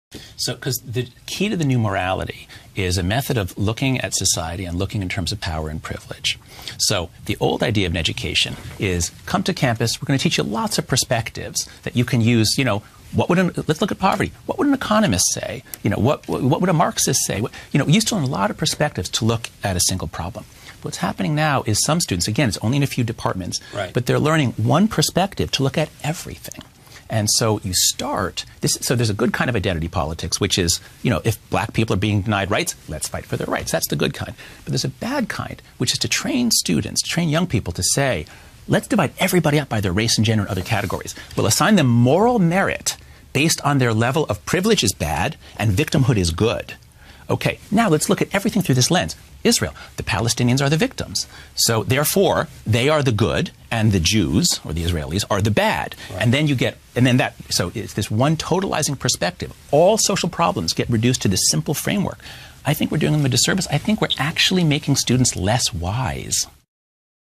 Jonathan Haidt - college campus SJW morality
Jonathan Haidt explains (in 1 minute) the major flaw in the morality of Social Justice Warriors on today's college campuses.